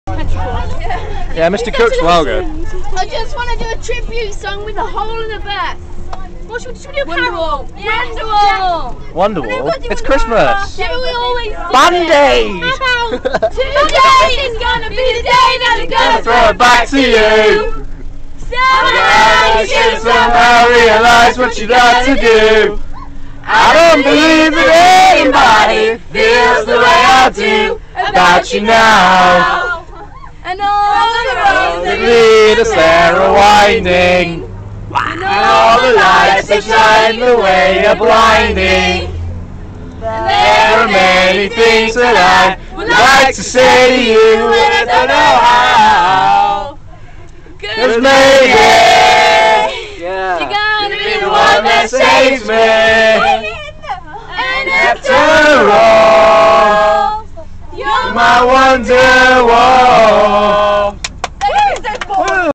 Bus home
Rockin our way along the road home. Its Christmaaaaaaas!!!
Yeah the words are in the wrong order but we dont give a shit.